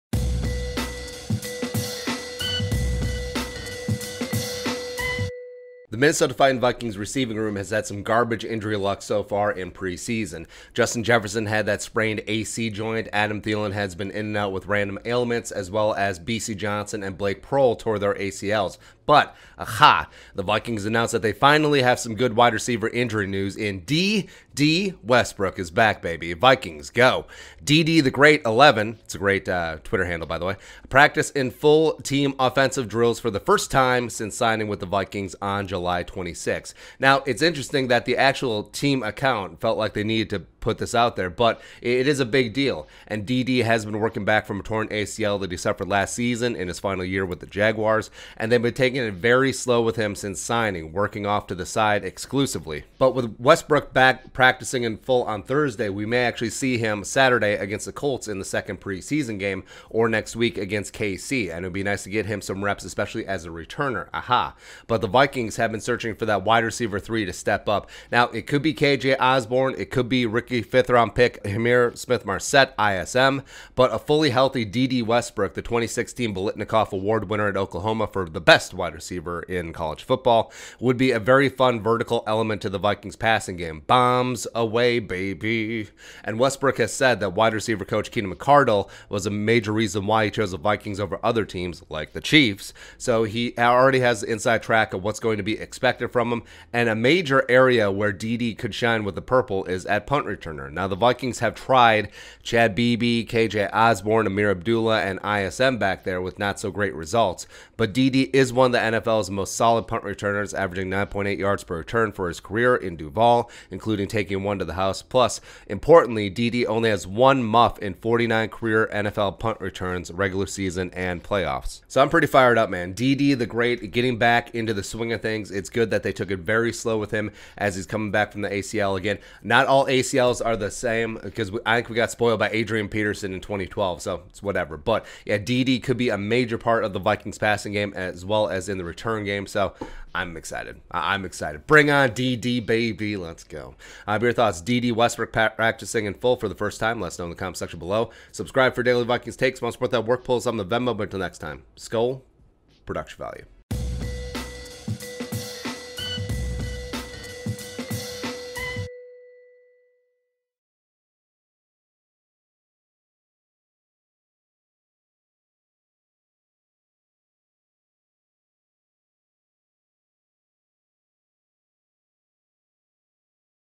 Dede Westbrook Practices In Full For First Time as a Minnesota Viking 🔥🔥🔥
Veteran speedster wide receiver/return man Dede Westbrook practiced in full for the first time as a Minnesota Viking as the squad eases him back from his torn ACL.